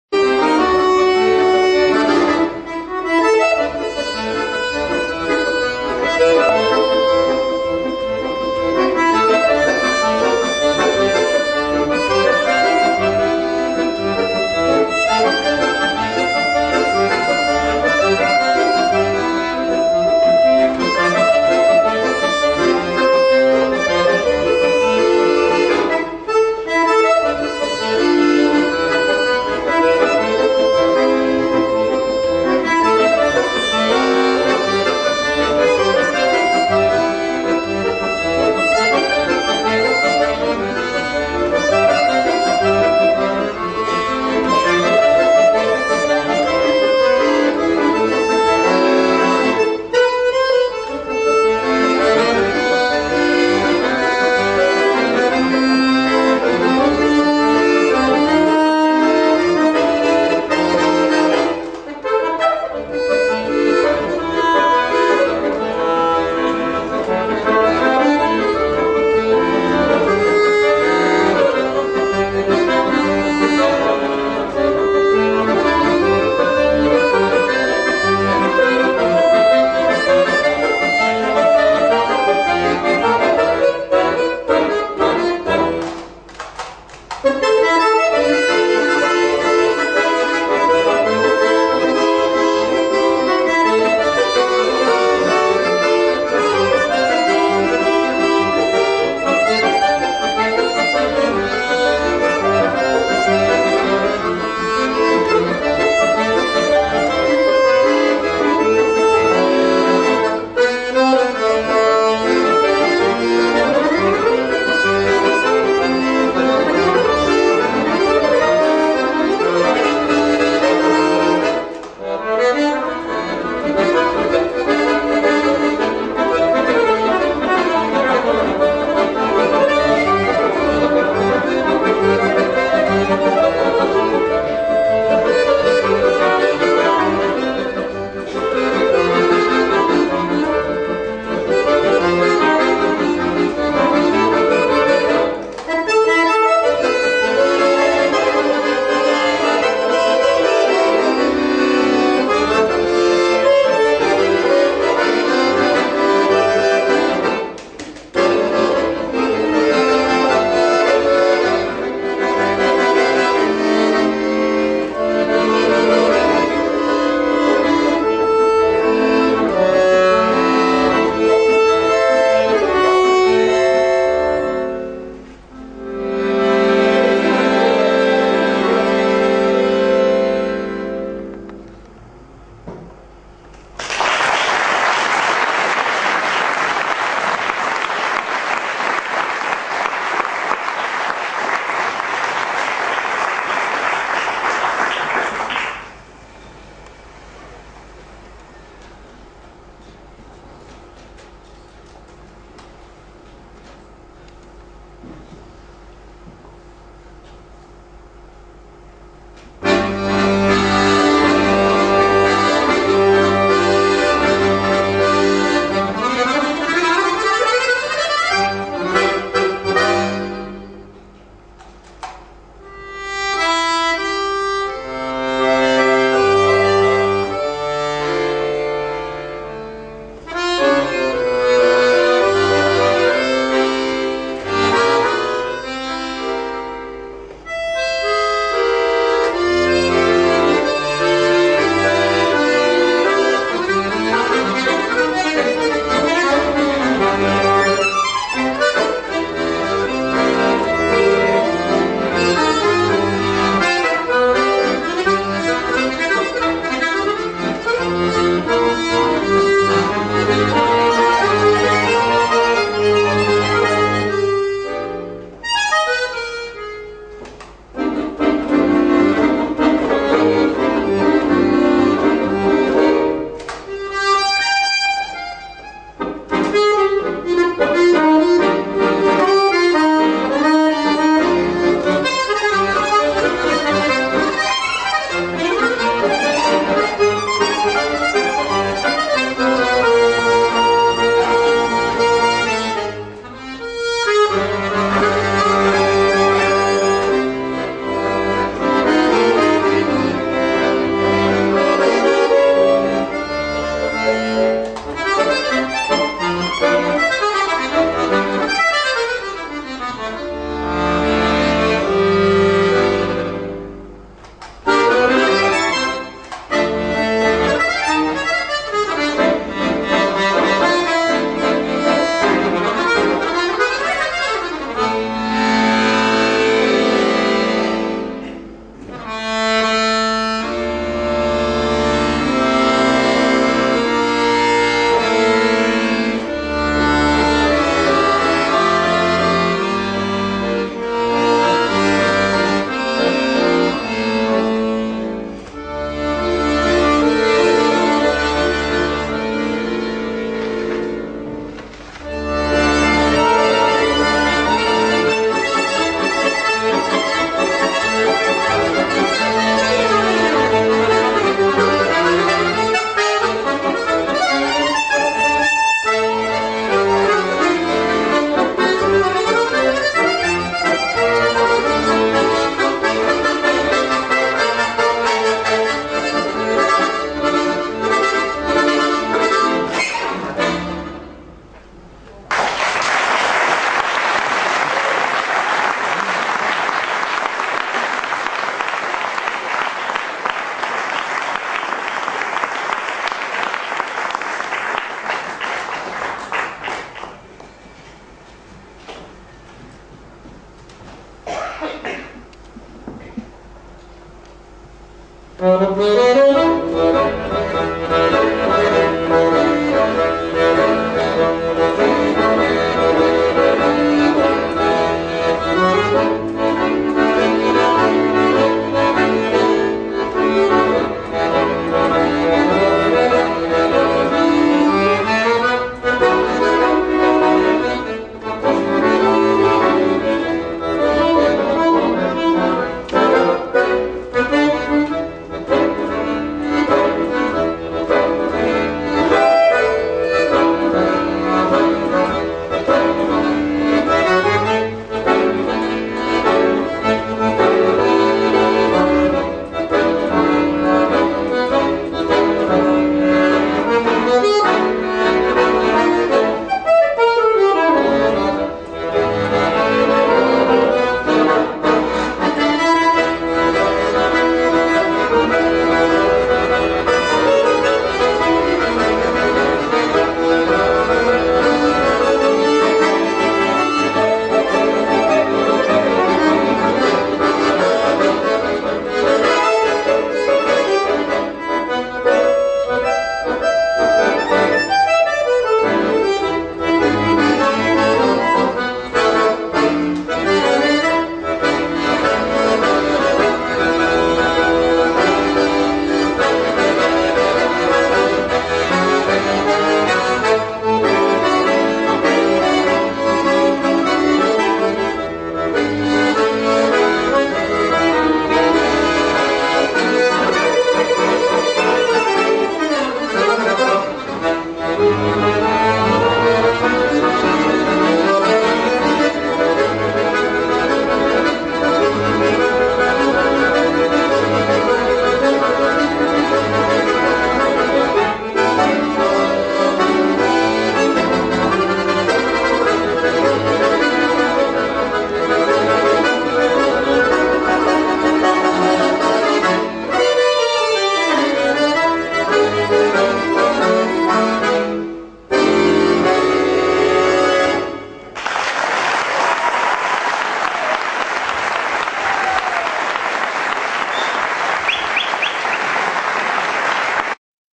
Maxime D'Errico France Senior Variete 1st Round
Confédération Mondiale de l'Accordéon
Trophée Mondial de l'Accordéon 2015 SWITZERLAND Martigny